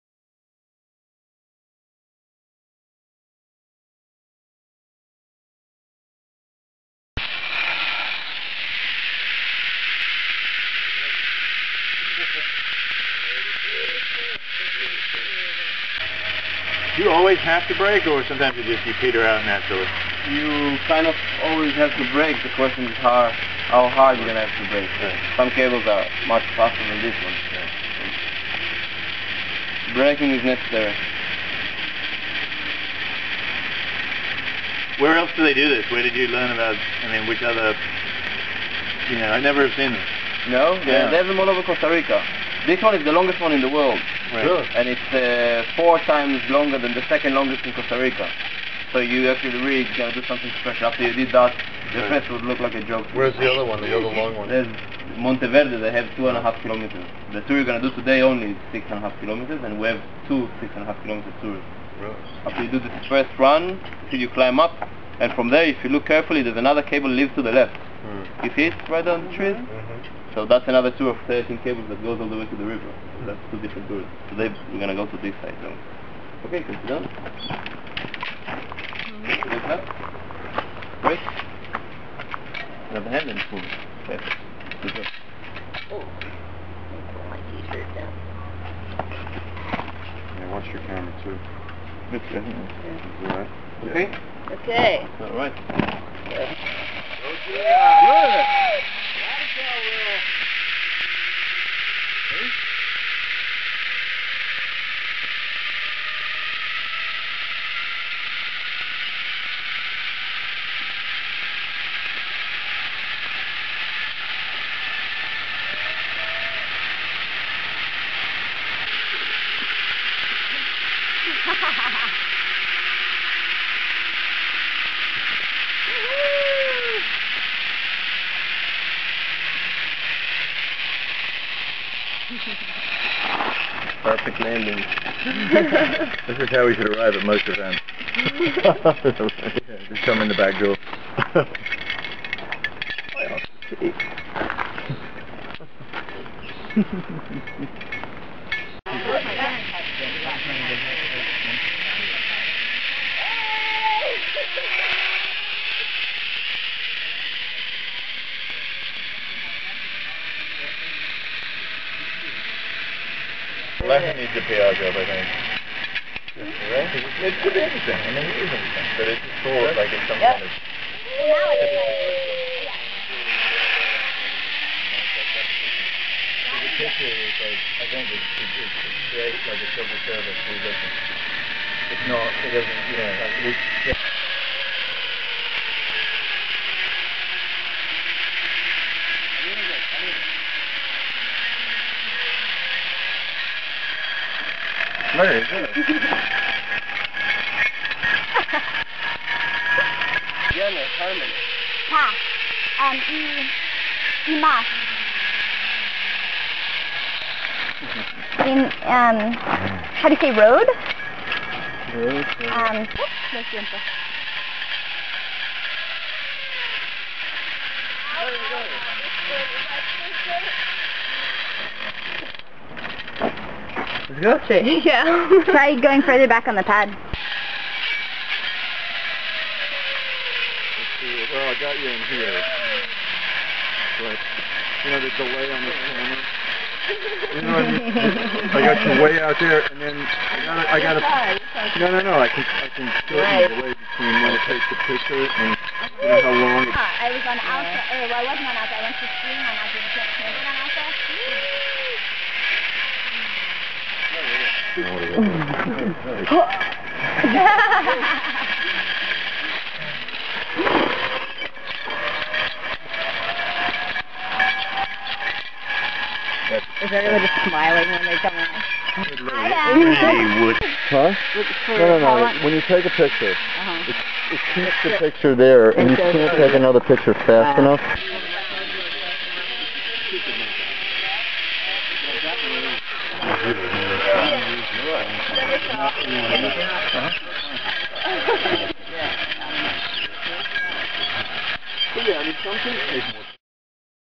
Costa Rica Zip Line Canopy Tour (July 6, 2007)
In July 2007 I was on vacation in Nosara, Costa Rica and went with a group for a canopy tour.  We went on half of the tour's total length, 6.5 km of the complete set of runs that stretch over 12 km.